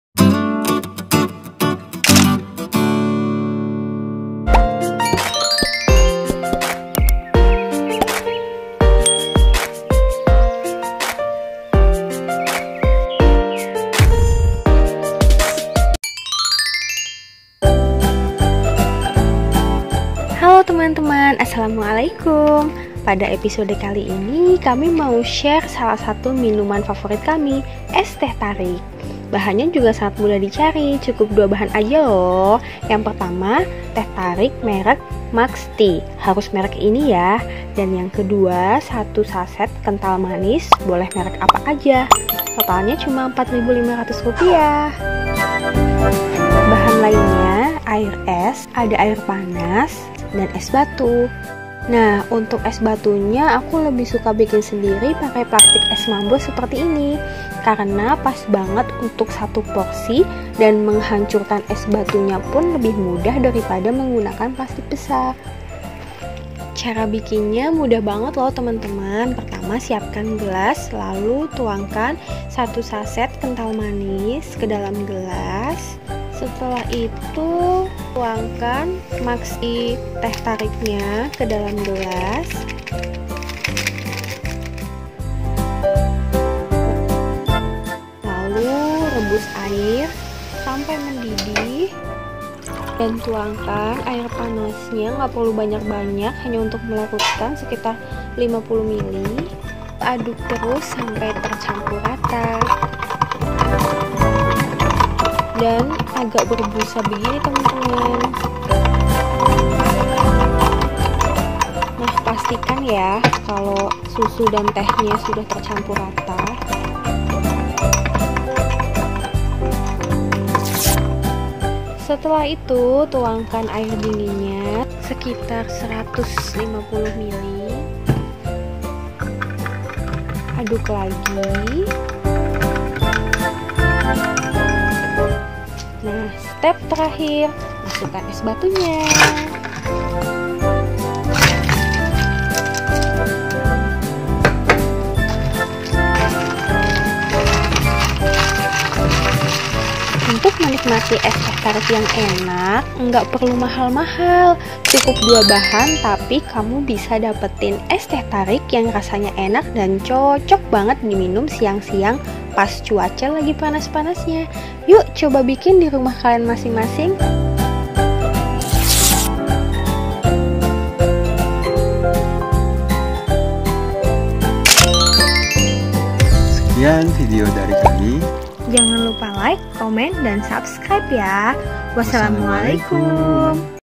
RESEP ES TEH TARIK HARGA HEMAT RASA PREMIUM CUMA 2 BAHAN!!
Pada episode kali ini kami mau Share resep ES TEH TARIK kesukaan kami,  cuma pakai 2 bahan dengan harga SANGAT MURAH TAPI RASA GA MURAHAN..

Mau tau cara bikinnya?  Gampaaang bangeet pokoknya, ayo simak video nya sampai habis.
Semoga bermanfaat ya untuk kalian semua.

Untuk resep-resep masakan dan minuman lainnya,  bisa Follow Cookpad ku juga ya

Cookpad : hervita nia

Thankyou.